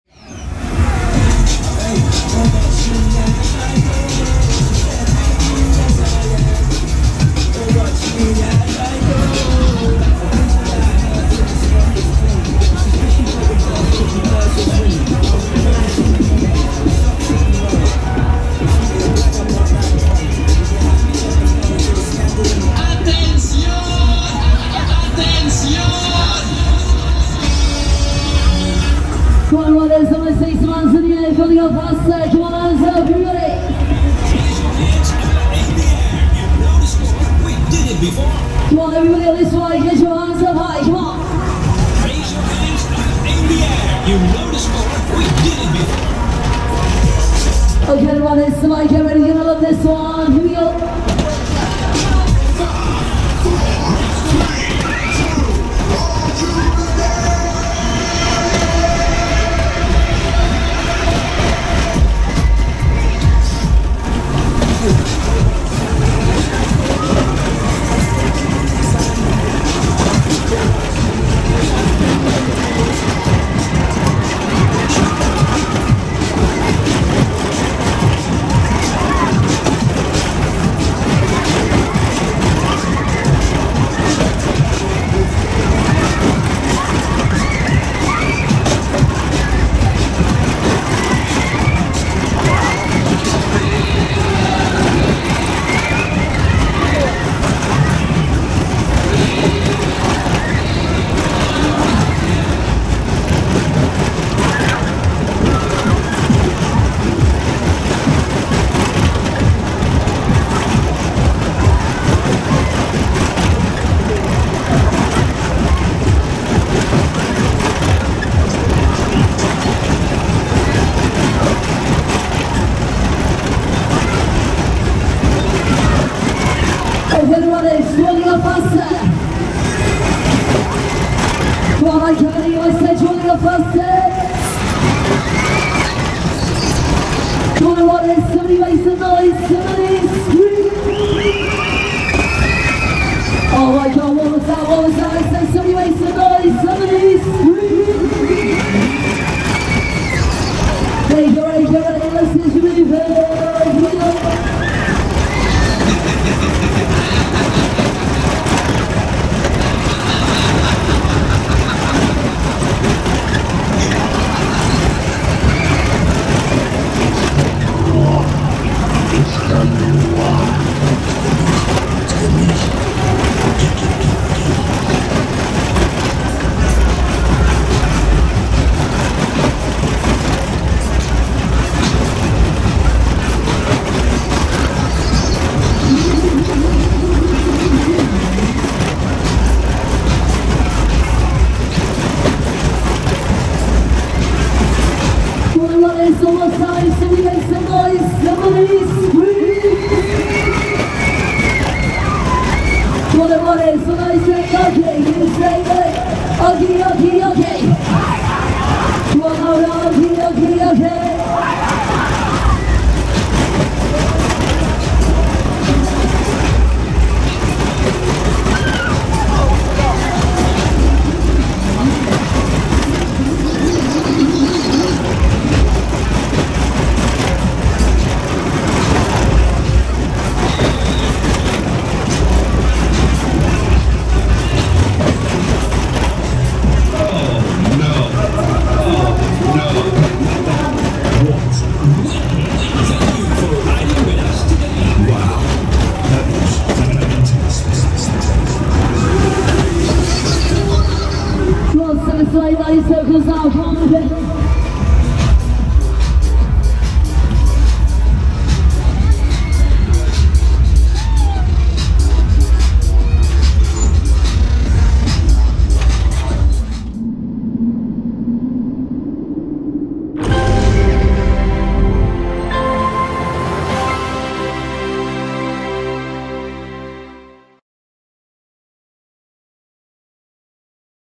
Extreme Matterhorn - James Rogers (Onride) @ Tewkesbury Mop Fair 2024
Onride footage of "Extreme" Sobema Matterhorn at Tewkesbury Mop Fair 2024.

Held on the 9th - 10th October across various streets in the town centre of Tewkesbury in Gloucestershire with James Danter & Sons at lessee.